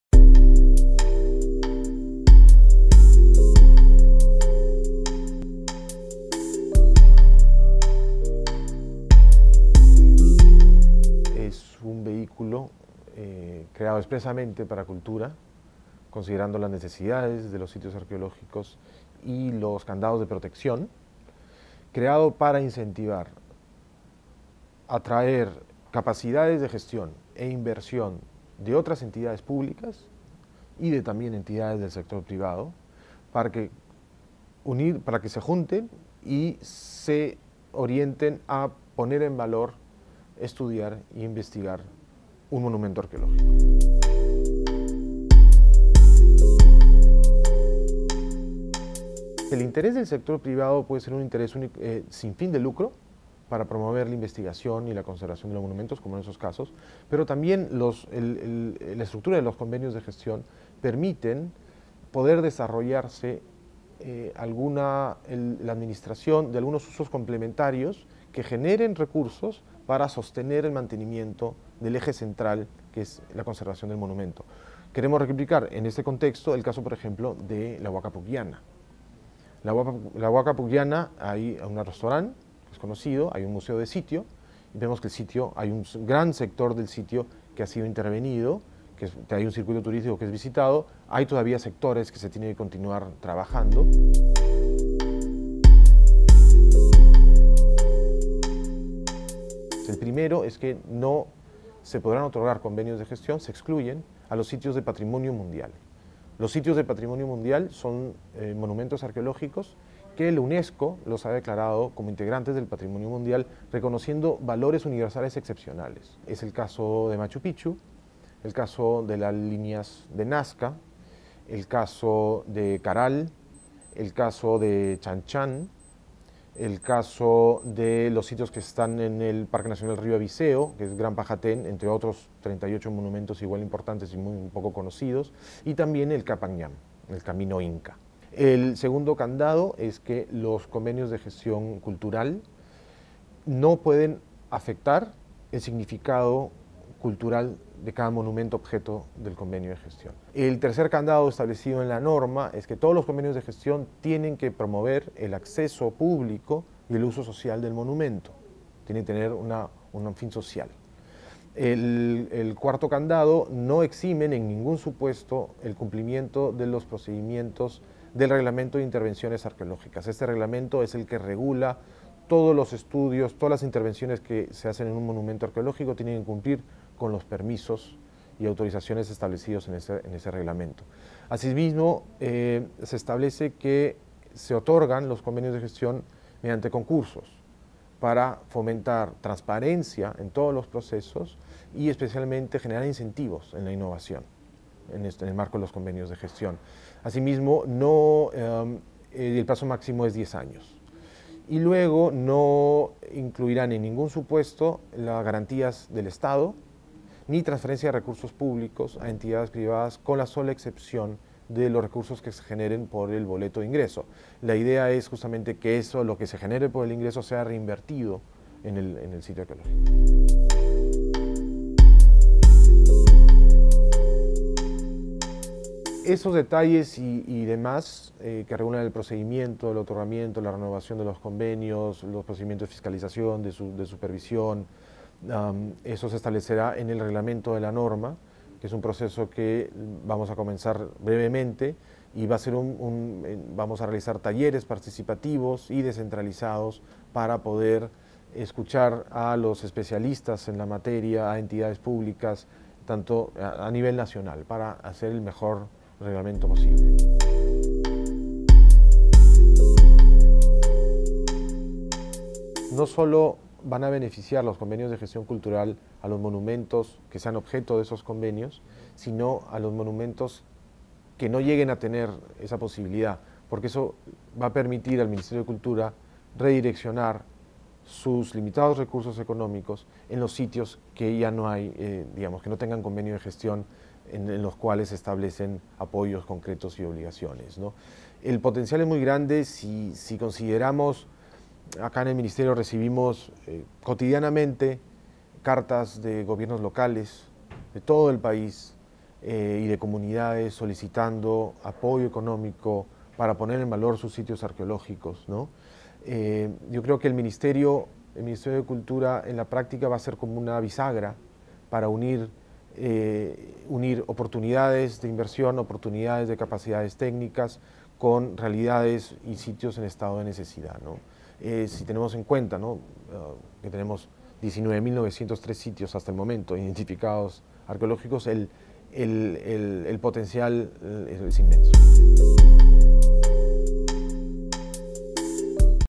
Viceministro: "Queremos replicar el caso de la Huaca Pucllana"
Juan Pablo de la Puente, Viceministro de Patrimonio Cultural, explica la nueva propuesta del Ministerio de Cultura: convenios que permiten a los privados administrar centros arqueológicos.